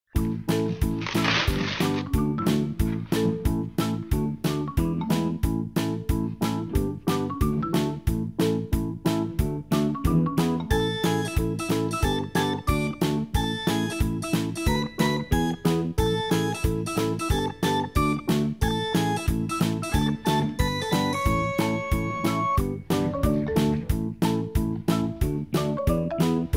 Frenchie Reviews Doggie Backpack
Frenchie is out for a stroll and he's got his favorite new backpack!